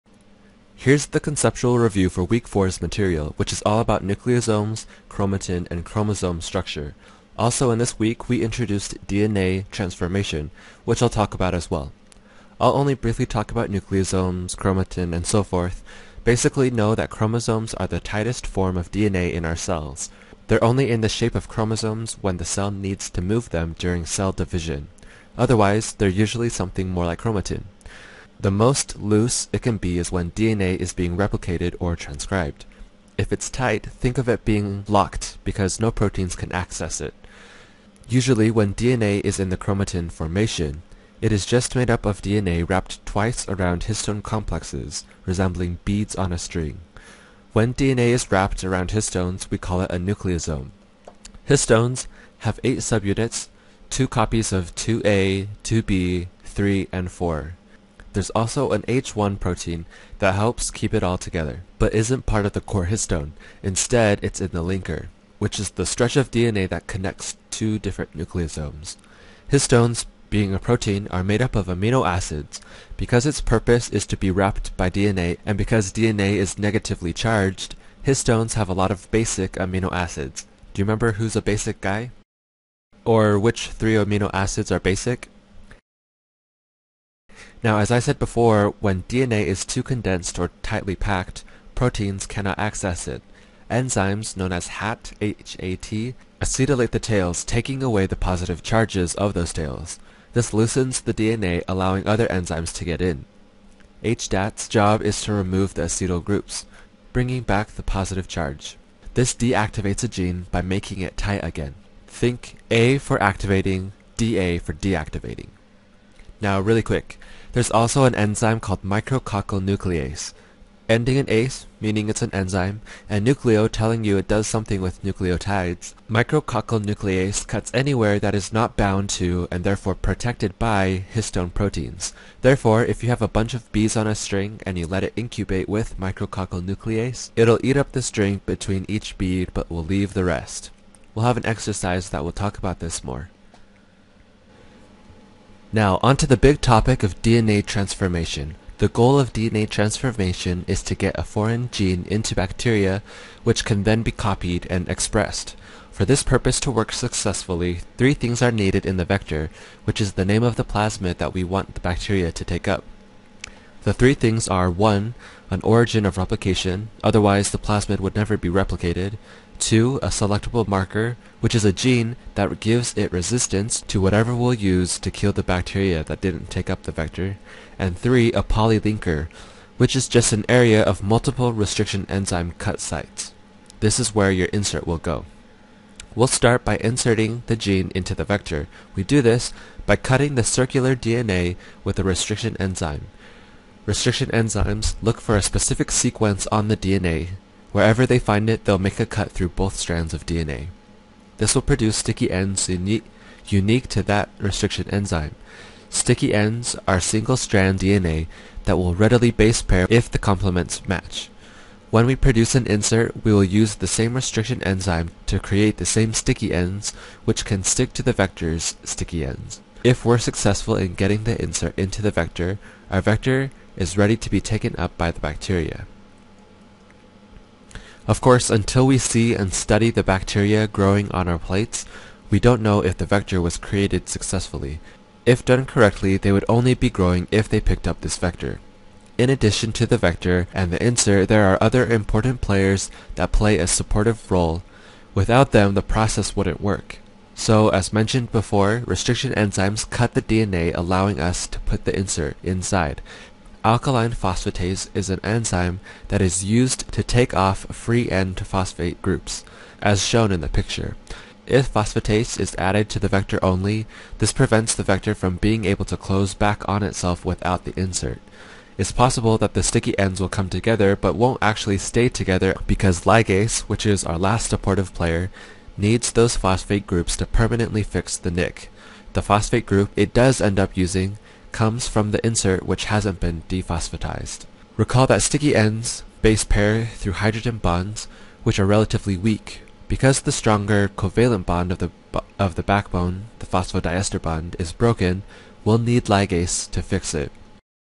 Week 4 - Nucleosome, Chromatin, Chromosome Structure + DNA Transformation | Part 1 - Concepts
Online TA review for Week 4's material.